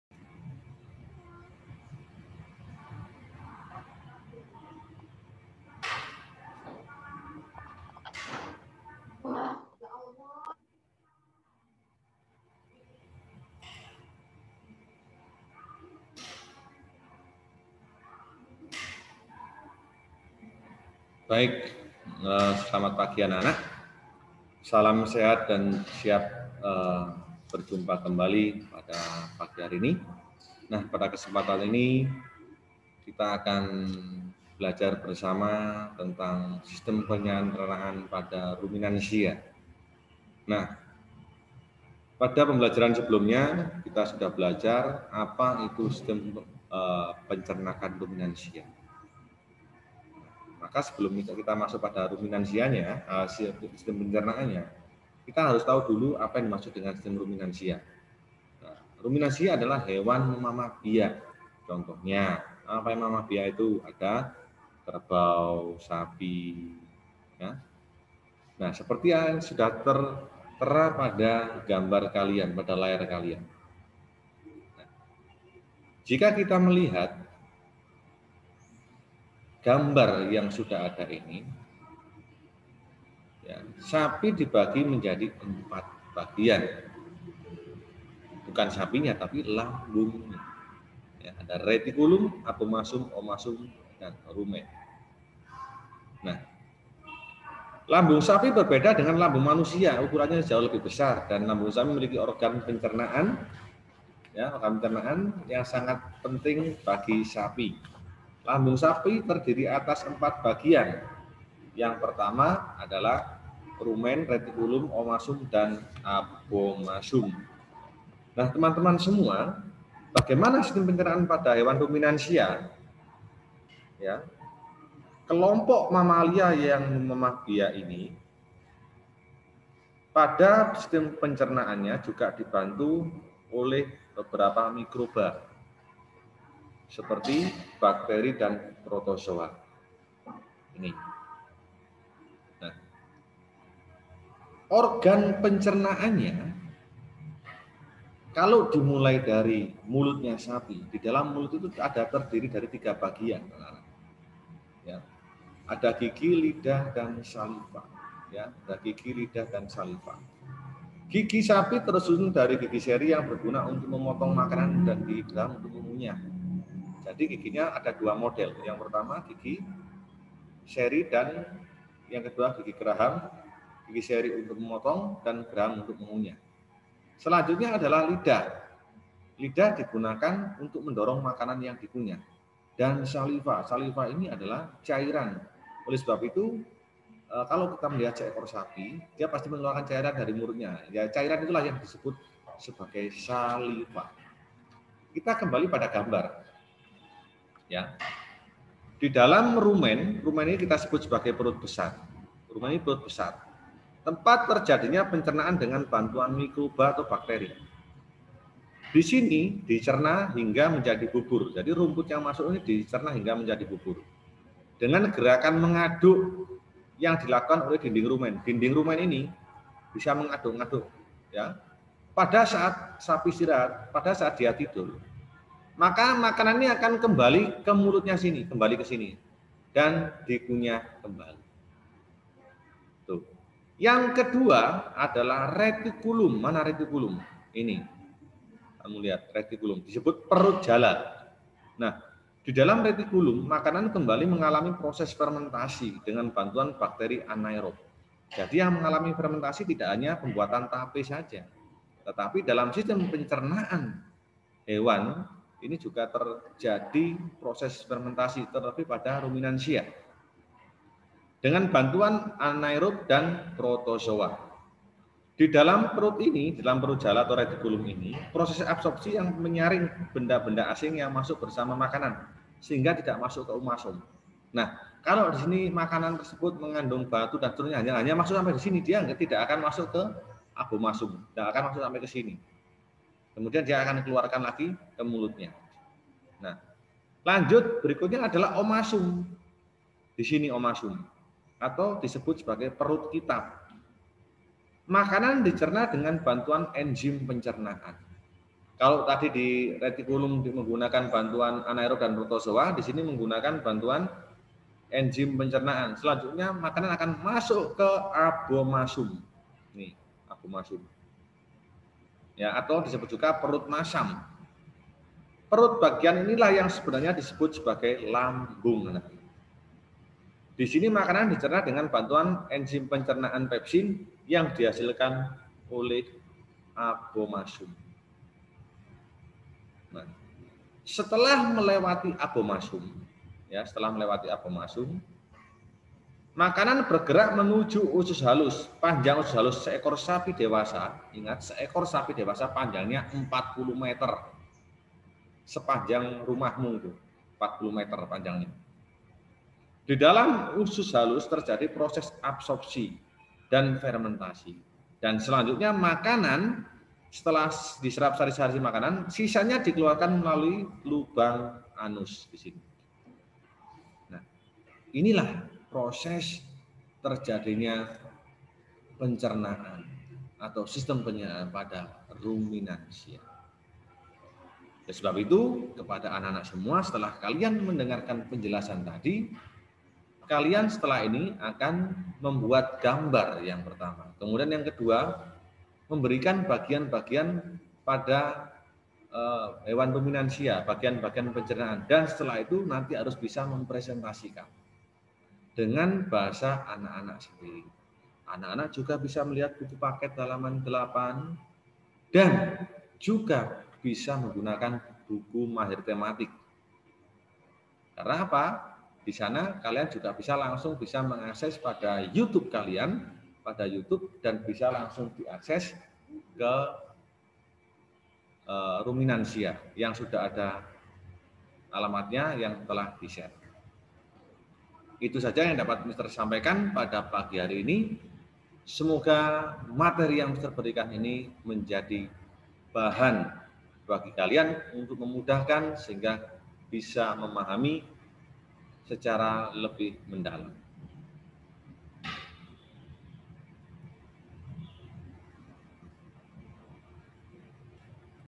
Sistem Pencernaan Ruminansia (Tema 3 Kelas 5)
Video pembelajaran daring. Materi kelas 5 tema 3 sub tema 1 tentang sistem pencernaan ruminansia